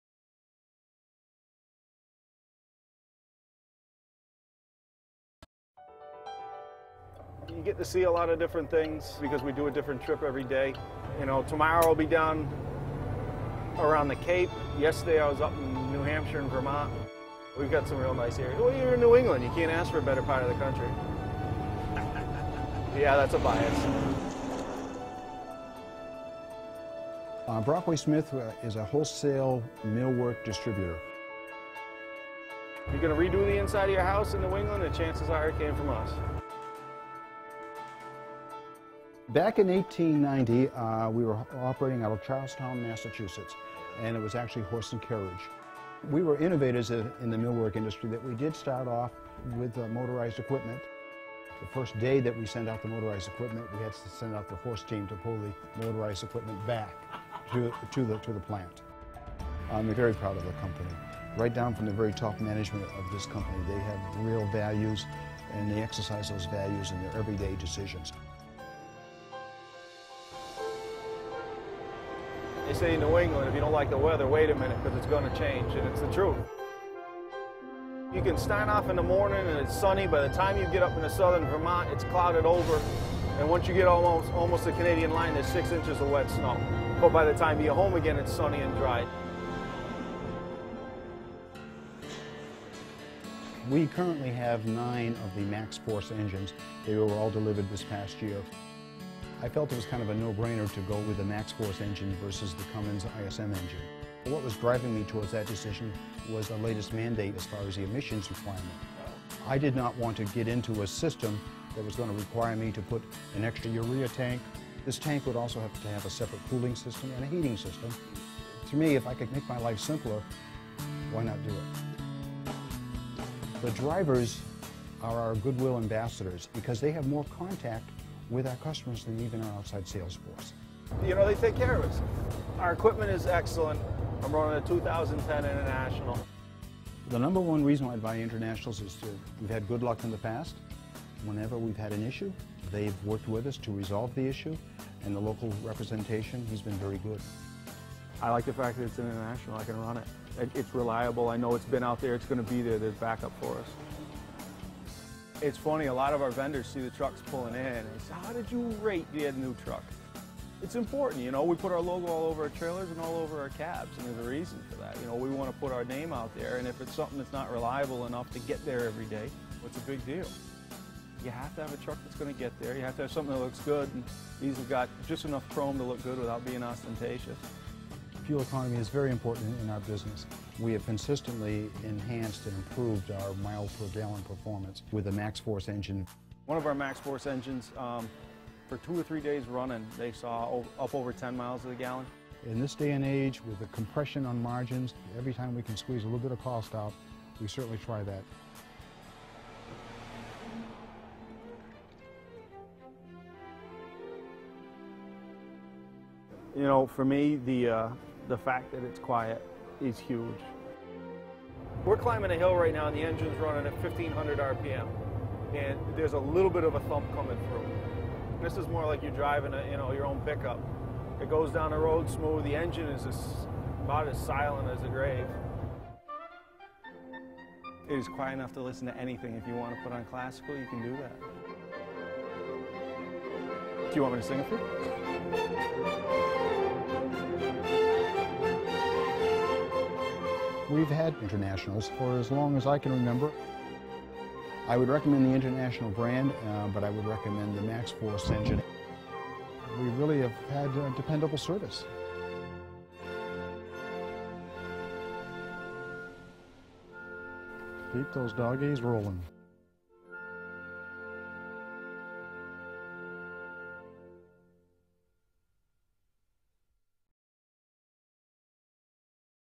International Trucks - MaxxForce
Meet Al Dirth, GM of Brockway-Smith, a manufacturing company shown delivering their products by International® TranStar® trucks powered by MaxxForce® 11 engines. Al & driver Mike, share why this truck & engine combo is simply the best.

I shot this in HD with John Komnenich for STORY out of Chicago.
Bill Cusick Productions at BillCusick.com